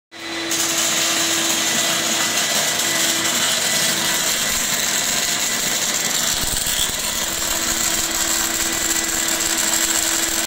EWM Taurus S 451 - Cjays Welding Equipment
Test Welding of The EWM Taurus S 451 Welding Machine - Cjays Welding Equipment @Ewm-group